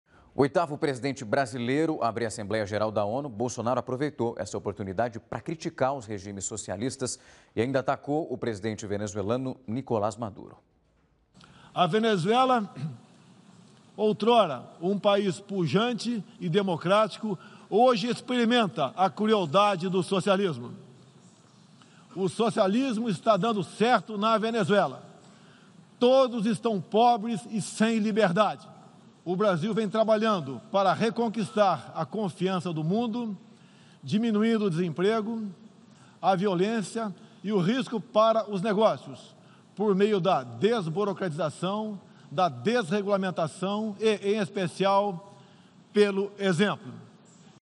Bolsonaro faz críticas ao socialismo
Em seu discurso de estreia na Assembleia-Geral da ONU (Organização das Nações Unidas), o presidente Jair Bolsonaro fez críticas aos regimes socialistas.